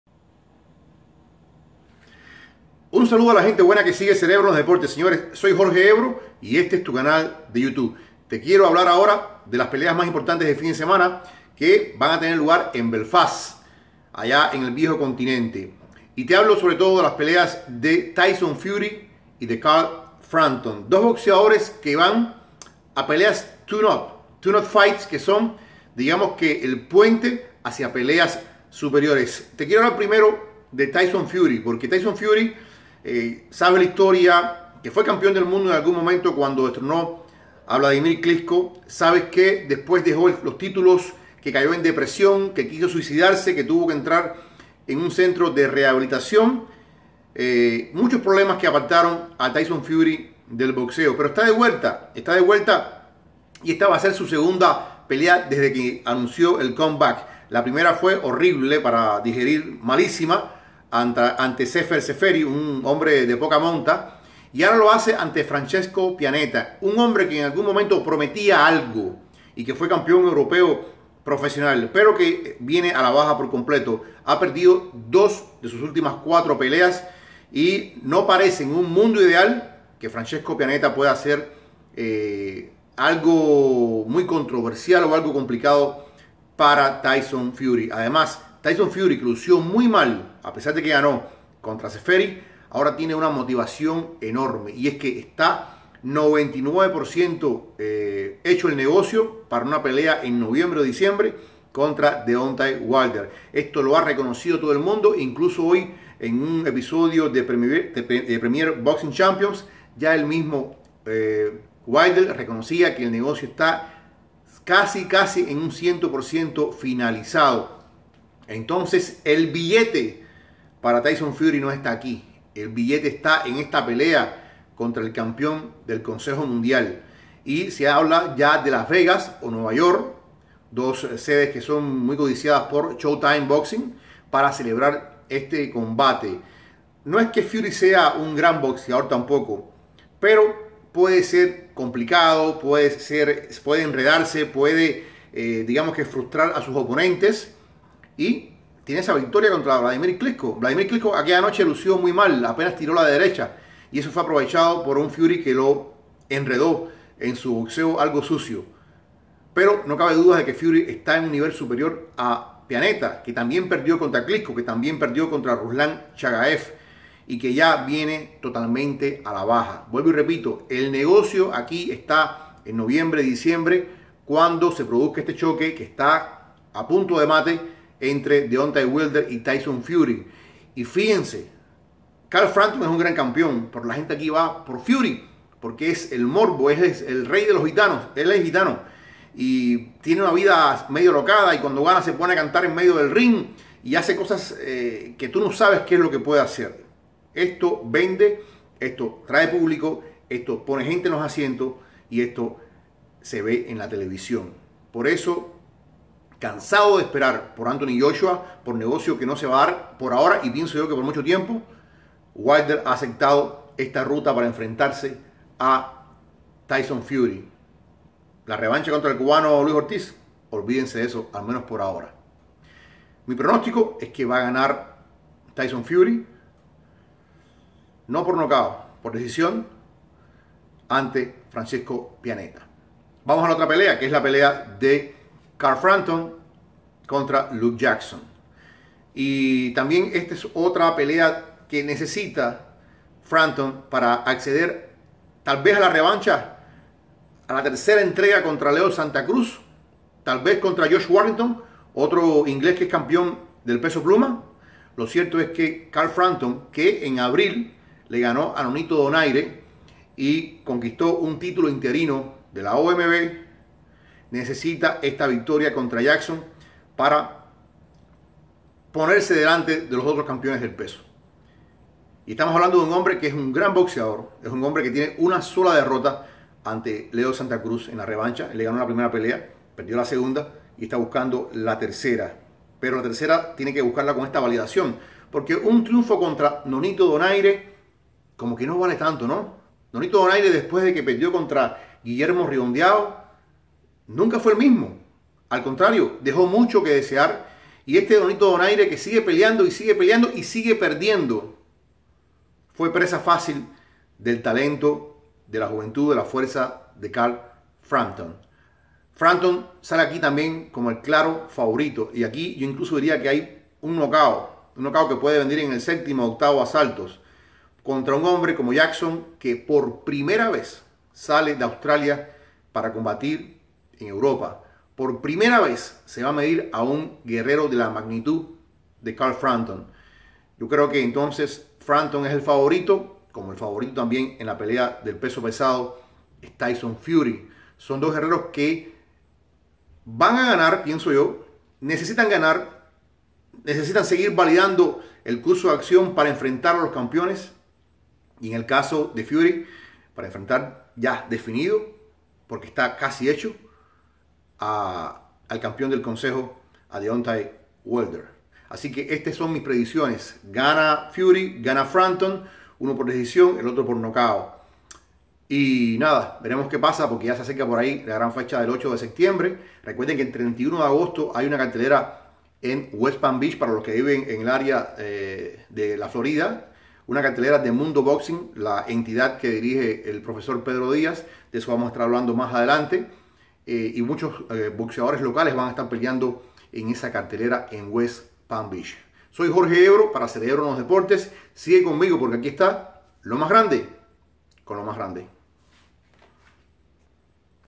Fury y Frampton, obligados a ganar para ir por premios mayores
Ambos pelean este sábado.